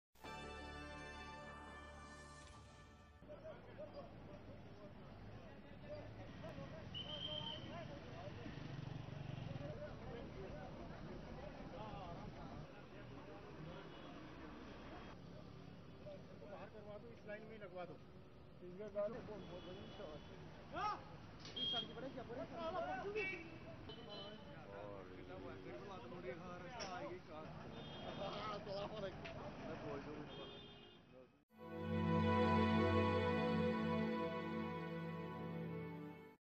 BSF Deployed in Kashmir to Maintain Vigil on Law And Order
Border Security Force (BSF) has been deployed in the Kashmir Valley to keep a check on the law and order situation, following the gun-down of top Hizbul Mujahideen commander Burhan Wani. Curfew and shutdown continued in the Kashmir Valley for the 47th consecutive day with all educational institutions, shops, public transport and other businesses being closed since July 9.